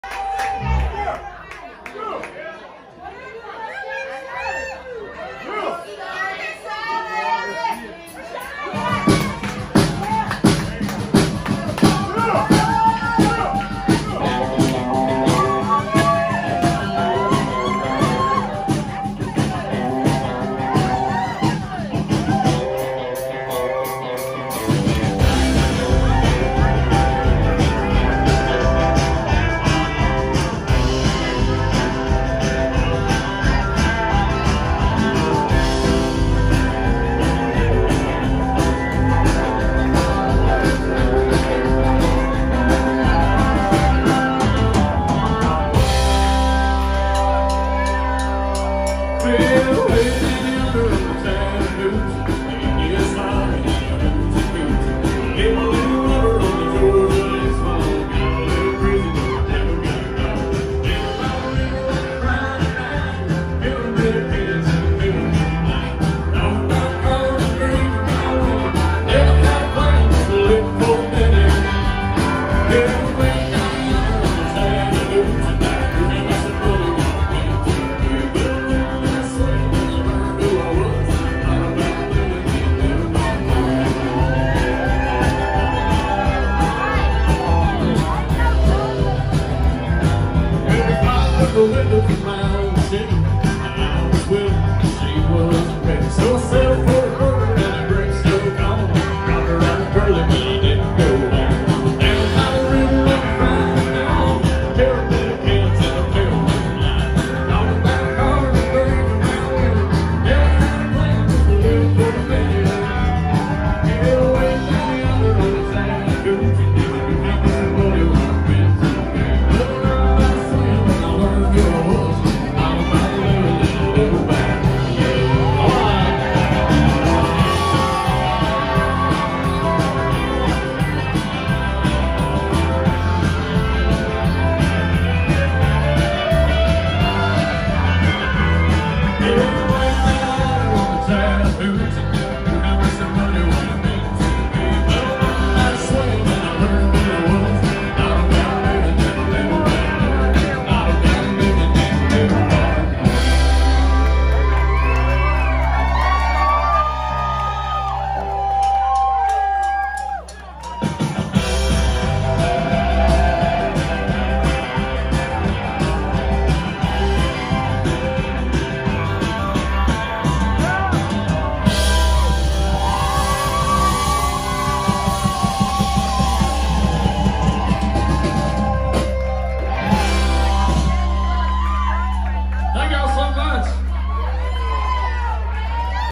Alex Key at Murphy’s on the Ocean, North Myrtle Beach, May 7, 2021
A fun night at MOTO!  Alex Key and the Locksmiths.